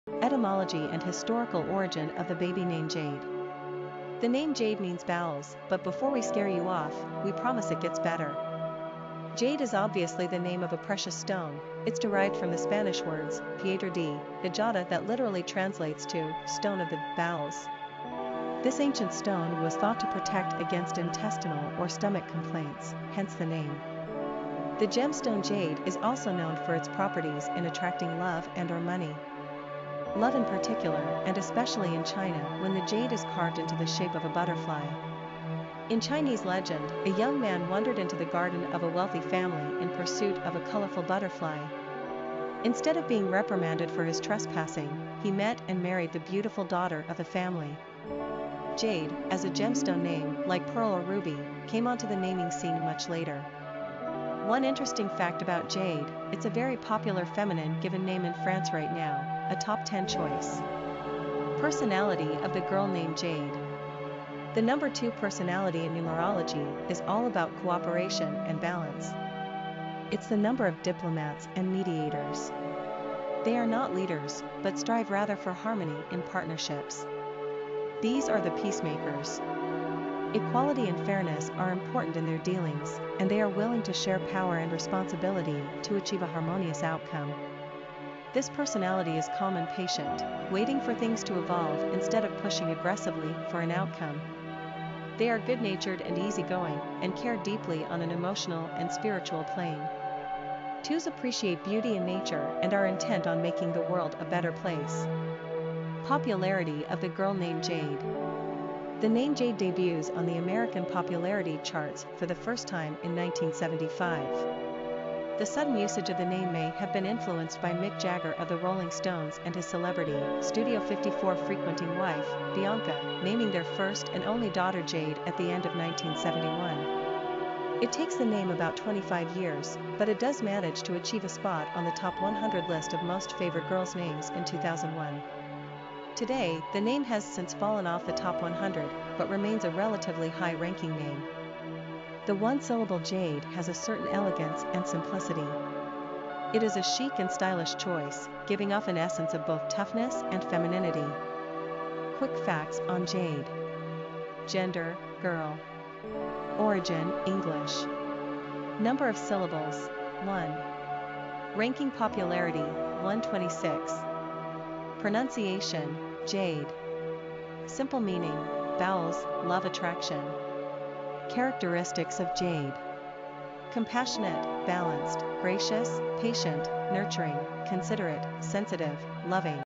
Jade - Girl Baby Name Meaning, Origin and Popularity
The name Jade means ‘bowels’ but before we scare you off, we promise it gets better.